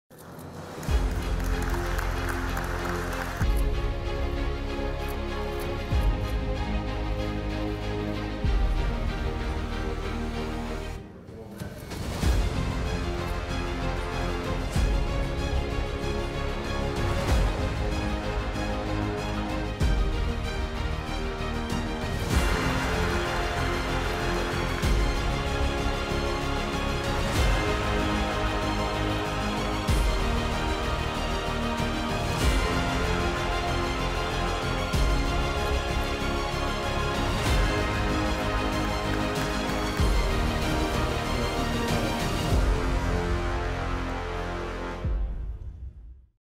رؤساء مجموعة دول الأنديز يجتمعون في ليما
يجتمع رؤساء بيرو وكولومبيا وبوليفيا والإكوادور لحضور اجتماع جماعة دول الأنديز، حيث تتولى بوليفيا الرئاسة المؤقتة للكتلة.